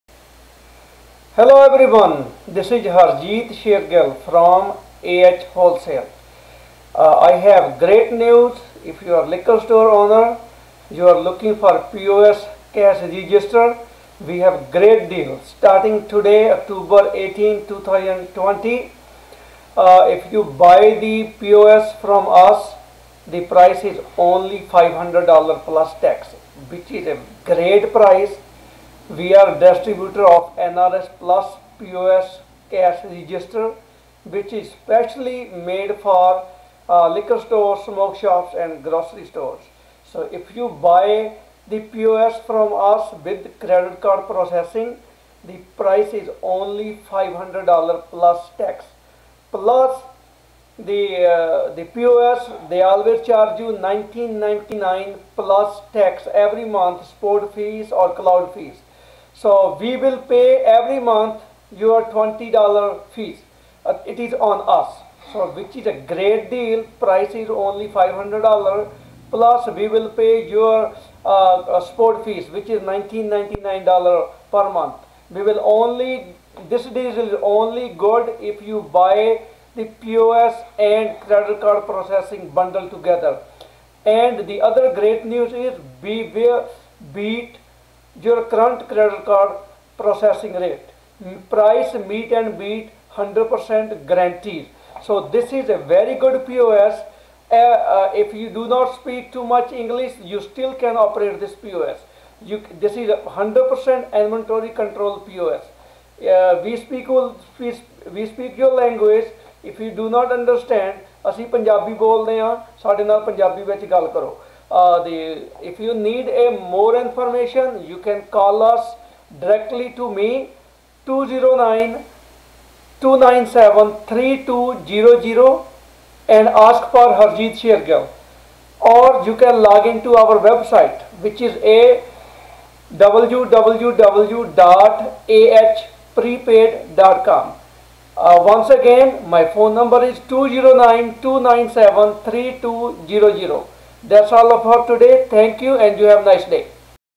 NRS Plus POS Cash Register Made For Liquor Stores-Smoke Shops
NRS Plus POS cash register specially made for liquor stores,smoke shops and grocery stores.It is a fully inventory control system with could base data call us for more information Whats app or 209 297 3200